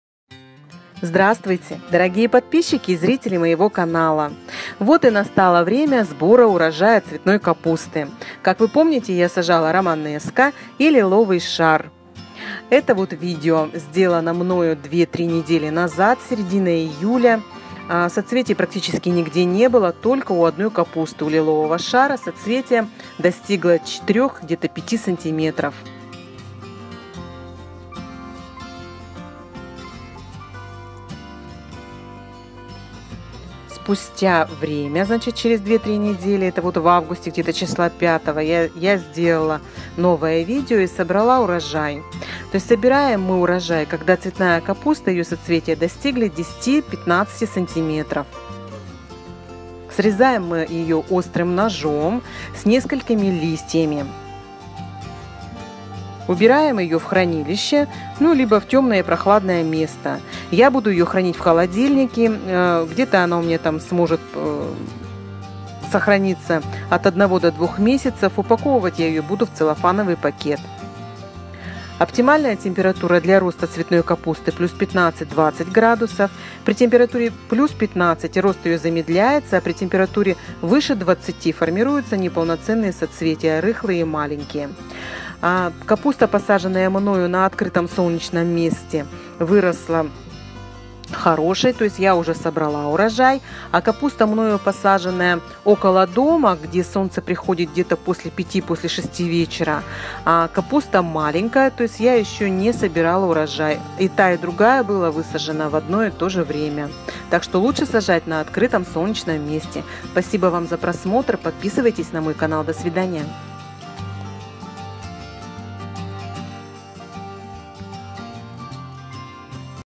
Когда и как проводить сбор урожая цветной капусты.
Оптимальная температура для роста цветной капусты + 15-20 градусов.
При температуре ниже +15 - её рост замедляется, а при температуре выше + 20 - формируются неполноценные соцветия, рыхлые и маленькие.
Уже с середины июля можно собирать урожай - это зависит от того какую капусту вы посадили - раннеспелую, среднеспелую или позднюю.
Проводим сбор урожая в сухую погоду до 19:00 часов вечера и нельзя проводить сбор капусты после дождя,  соцветие должно достигнуть 10-15 см в диаметре, срезаем острым ножом с несколькими нижними листьями. Убираем в хранилище или в холодильник, в ящик для овощей, предварительно упаковав в целлофановый пакет.